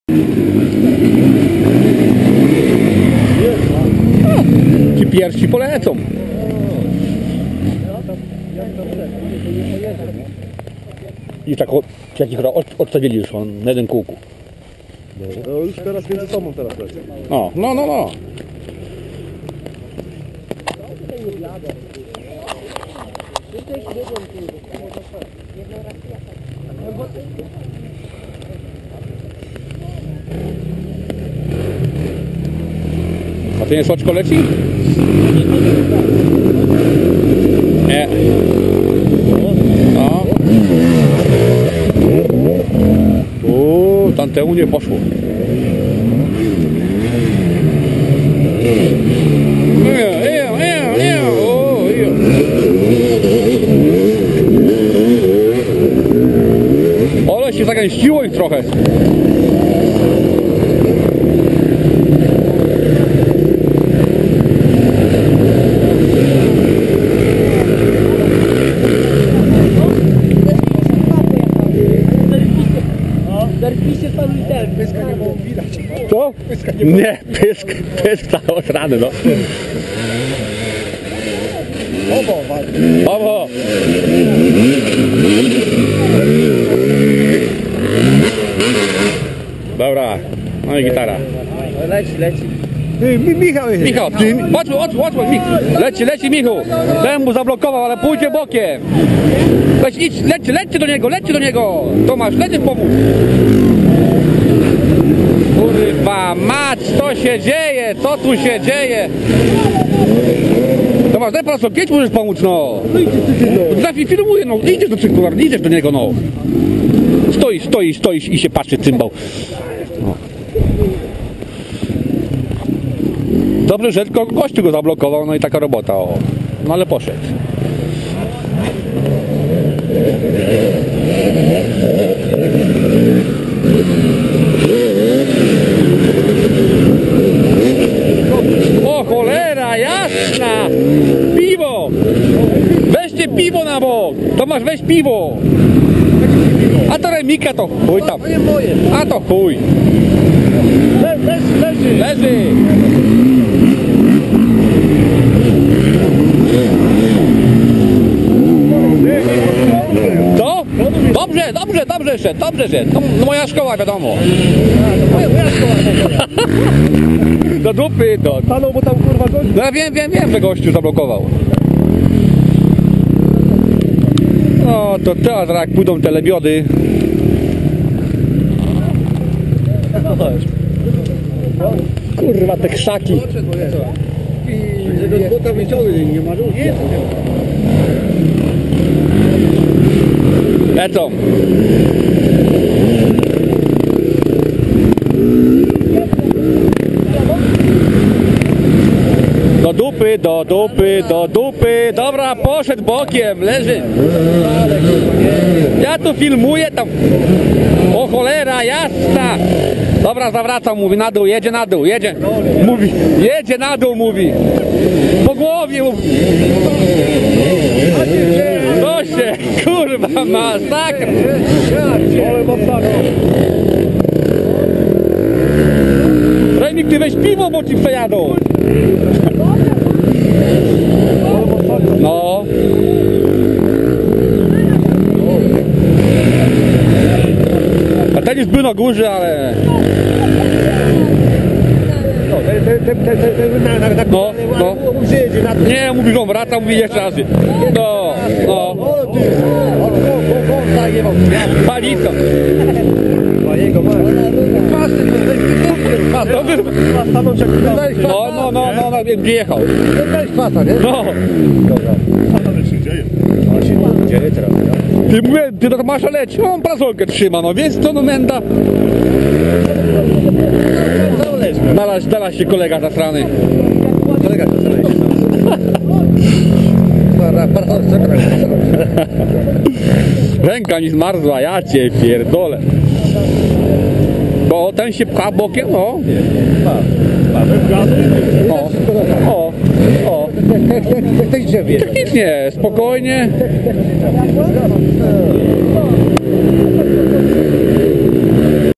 Podjazd, pierwsze okrążenie - Siedlęcin XXV Piknik Cross Country 15.04.2012
Podjazd na pierwszym okrążeniu, niezły korek. XXV Piknik Cross Country Siedlęcin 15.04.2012